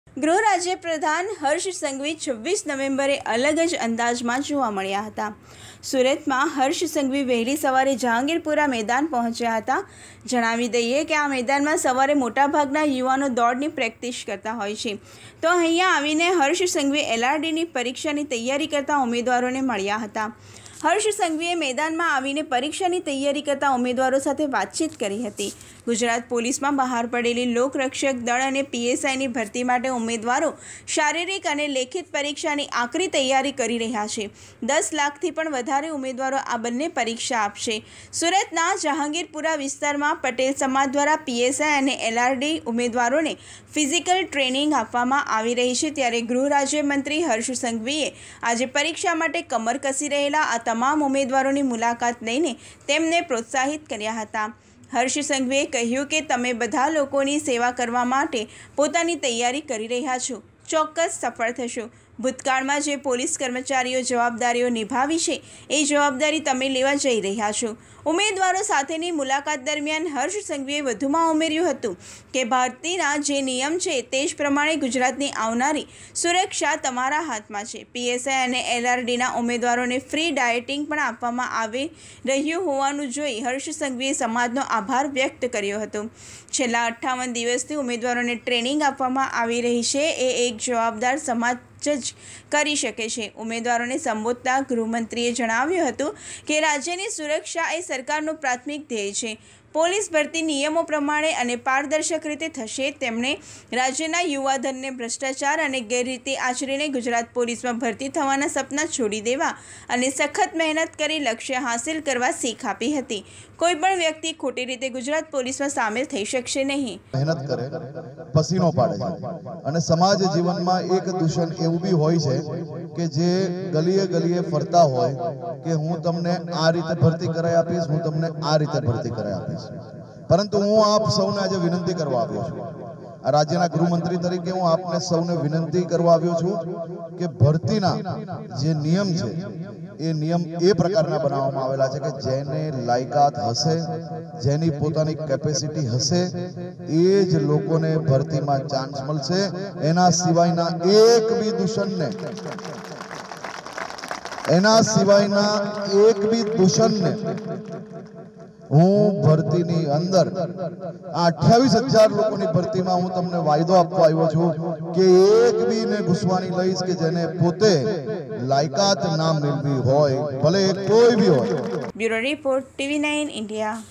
|TV9INDIA GUJARATI
ગૃહ રાજ્યપ્રધાન હર્ષ સંઘવી 26 નવેમ્બરે અલગ જ અંદાજમાં જોવા મળ્યા.
વહેલી સવારે LRD ઉમેદવારો કરી રહ્યા હતા પ્રેક્ટીસ, તેમનો જુસ્સો વધારવા પહોંચી ગયા હર્ષ સંઘવી
આ પરીક્ષા આપણા જીવનની છેલ્લી પરીક્ષા છે, એટલે મન પરથી કોઈ ભાર હોય તો ઉતારી લેજો: હર્ષ સંઘવી
'લાયકાત વિનાનાને ભરતીમાં ઘૂસવા નહીં દઉં, મારો વાયદો છે, ઉમેદવારોના માર્ગદર્શન માટે 4 શો આવશે'

ગૃહ રાજ્યપ્રધાન હર્ષ સંઘવી (Harsh Sanghavi) 26 નવેમ્બરે અલગ જ અંદાજમાં જોવા મળ્યા. સુરતમાં (Surat) હર્ષ સંઘવી વહેલી સવારે જહાંગીરપુરા મેદાન પહોંચ્યા હતા. જણાવી દઈએ કે આ મેદાનમાં સવારે મોટાભાગના યુવાનો દોડની પ્રેક્ટીસ કરતા હોય છે. તો અહીંયા આવીને હર્ષ સંઘવી LRD ની પરીક્ષાની (LRD Exam) તૈયારી કરતા ઉમેદવારોને મળ્યા હતા. હર્ષ સંઘવીએ મેદાનમાં આવીને પરીક્ષાની તૈયારી કરતા ઉમેદવારો સાથે વાતચીત કરી હતી.

ગુજરાત પોલીસમાં બહાર પડેલી લોકરક્ષક દળ અને PSIની ભરતી માટે ઉમેદવારો શારીરિક અને લેખિત પરીક્ષાની આકરી તૈયારી કરી રહ્યા છે. 10 લાખથી પણ વધારે ઉમેદવારો આ બંને પરીક્ષા આપશે. સુરતના જાંગીપુરા વિસ્તારમાં પટેલ સમાજ દ્વારા PSI અને LRD ઉમેદવારોને ફિઝિકલ ટ્રેનિંગ આપવામાં આવી રહી છે. ત્યારે ગૃહ રાજ્યમંત્રી હર્ષ સંઘવીએ આજે પરીક્ષા માટે કમર કસી રહેલા આ તમામ ઉમેદવારોની મુલાકાત લઈને તેમને પ્રોત્સાહીત કર્યા હતા. હર્ષ સંઘવીએ કહ્યું હતું કે, તમે બધા લોકોની સેવા કરવામાં માટે પોતાની તૈયારી કરી રહ્યા છો, ચોક્કસ સફળ થશો, ભૂતકાળમાં જે પોલીસ કર્મચારીઓ જવાબદારીઓ નિભાવી છે એ જવાબદારી તમે લેવા જઈ રહ્યા છો.ઉમેદવારો સાથેની મુલાકાત દરમિયાન હર્ષ સંઘવીએ વધુમાં ઉમેર્યું હતું કે, ભરતીના જે નિયમ છે તેજ પ્રમાણે ગુજરાતની આવનારી સુરક્ષા તમારી હાથમાં છે. PSI અને LRDના ઉમેદવારોને ફ્રી ડાયટિંગ પણ આપવામાં આવે રહ્યું હોવાનું જોઈ હર્ષ સંઘવીએ સમાજનો આભાર વ્યક્ત કર્યો હતો. છેલ્લા 58 દિવસથી ઉમેદવારોને ટ્રેનિંગ આપવામાં આવી રહી છે. એ એક જવાબદાર સમાજ જ કરી શકે છે.ઉમેદવારોને સંબોધતા ગૃહમંત્રીએ જણાવ્યું હતું કે, ‘રાજ્યની સુરક્ષા એ સરકારનું પ્રાથમિક ધ્યેય છે. પોલીસ ભરતી નિયમો પ્રમાણે અને પારદર્શક રીતે થશે. તેમણે રાજ્યના યુવાધનને ભ્રષ્ટાચાર અને ગેરરીતિ આચરીને ગુજરાત પોલીસમાં ભરતી થવાના સપના છોડી દેવા અને સખ્ત મહેનત કરી લક્ષ્ય હાંસલ કરવા શીખ આપી હતી. કોઇપણ વ્યક્તિ ખોટી રીતે ગુજરાત પોલીસમાં સામેલ થઇ શકશે નહિ.